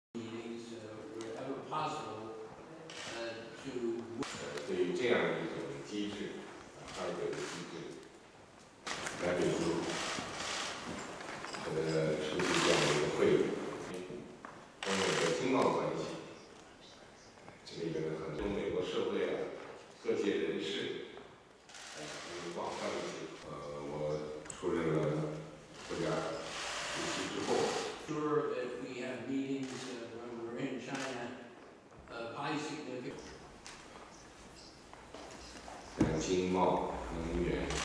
Xi leads China-US business leaders' dialogue
(17 Sep 2015) Chinese President Xi Jinping met with several entrepreneurs and former officials from the United States in Beijing on Thursday, just days ahead of his visit to the US.
Xi departs next week for a trip to Seattle, Washington DC and New York, with a summit planned with US President Barack Obama on September 25.
Xi has said he hopes the visit will deepen understanding and friendship between the two countries.
Discussions are expected on trade, North Korea's nuclear programme and Iran, among other issues.
The two sides are at odds however over allegations of Beijing-directed cyberattacks on the US and China's recent moves to assert its territorial claims in the South China Sea.